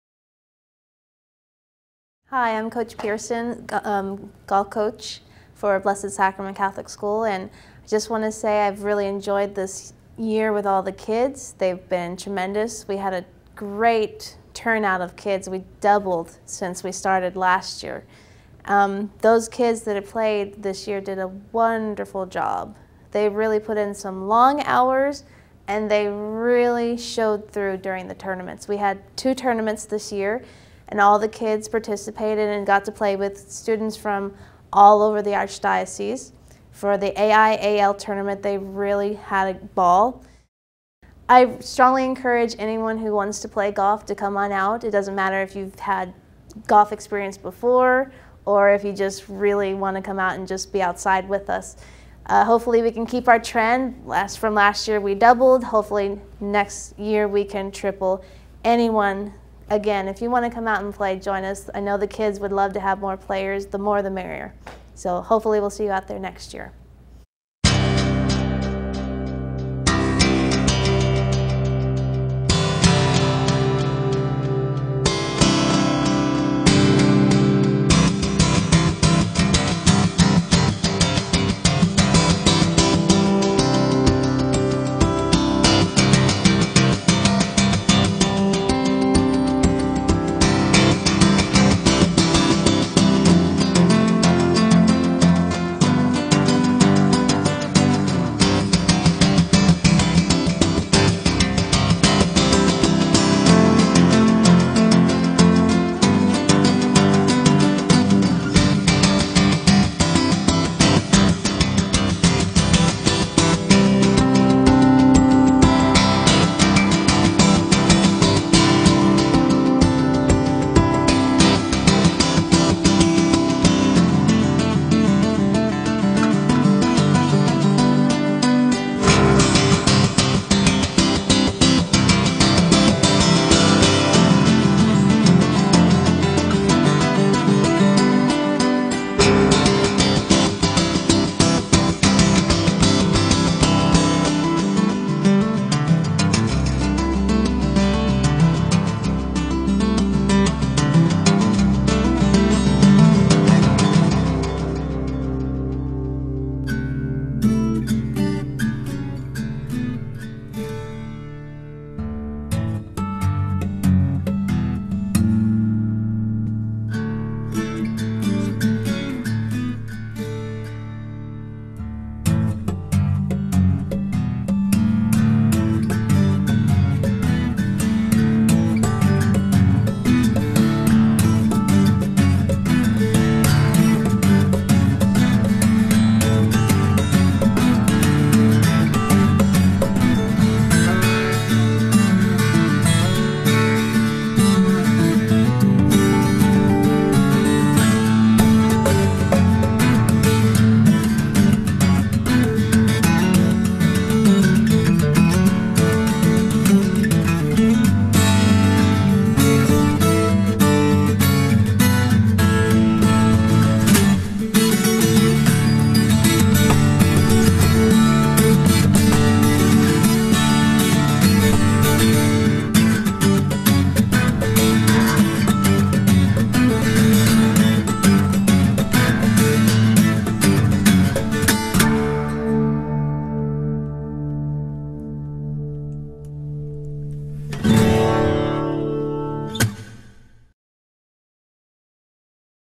Blessed Sacrament Golf 2008
This is the Golf segment of the Blessed Sacrament Bulldogs Sports Banquet video for school year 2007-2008.  Click on 'watch in high qualty' for better viewing.